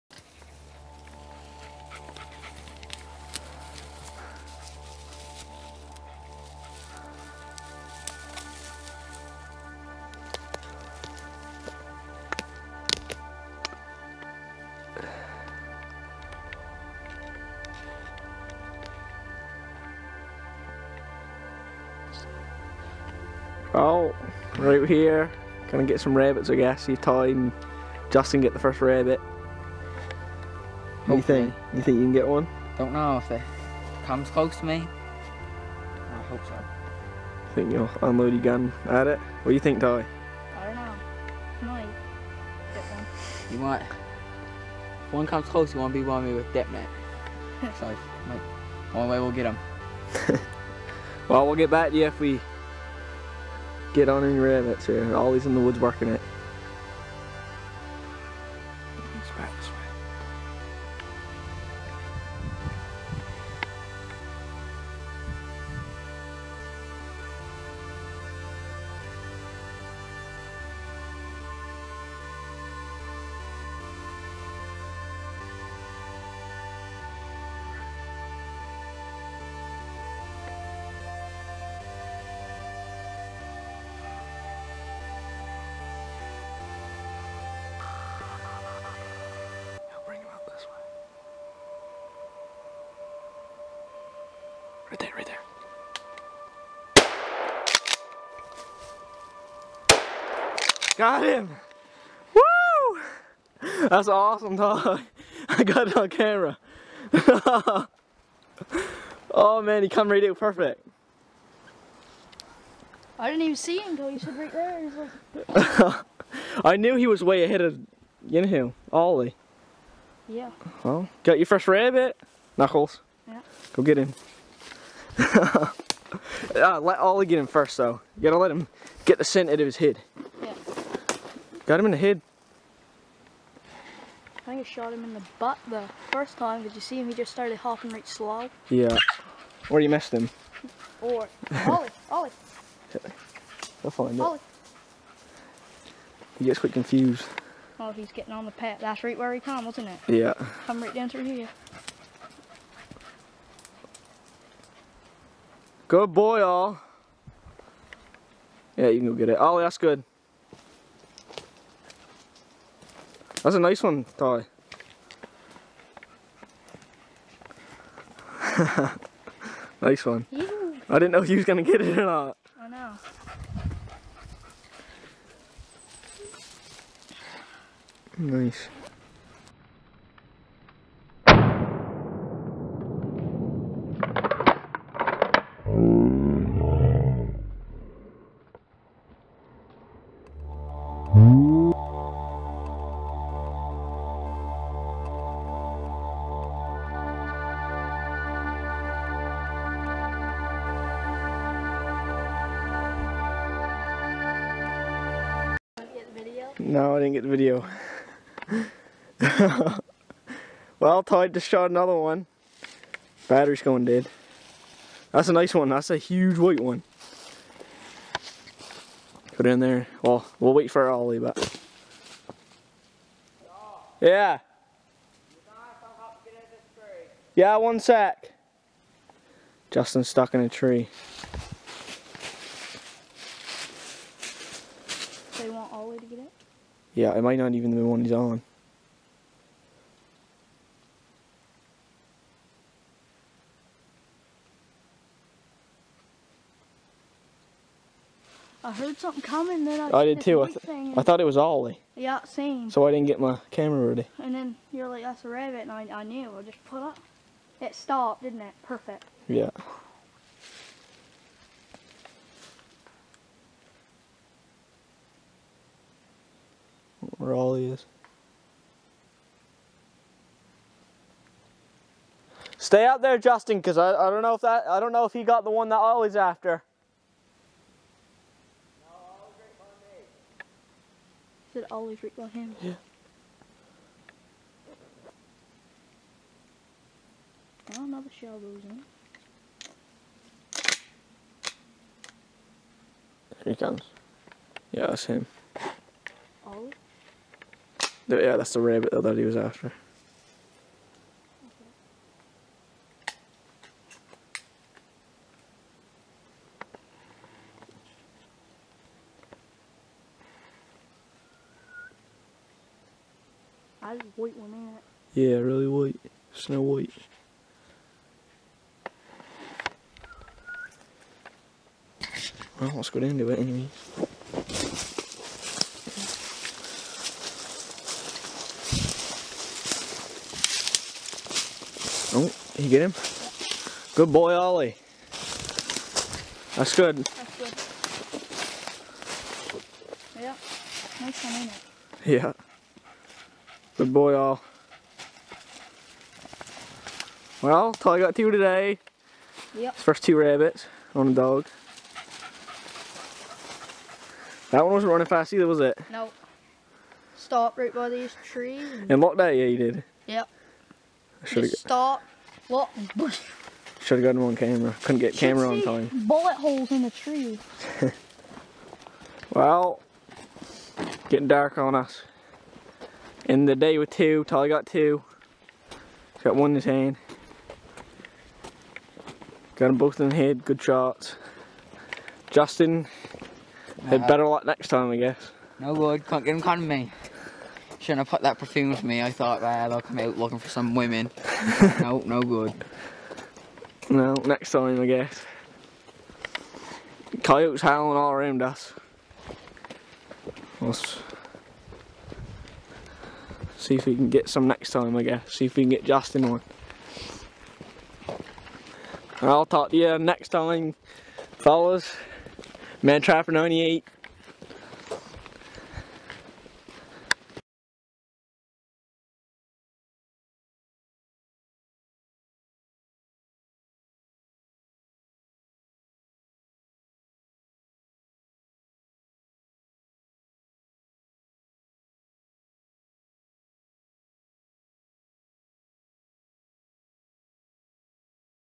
Hunting with a hound: ty's first rabbit
a good few hours of rabbit hunting payed off.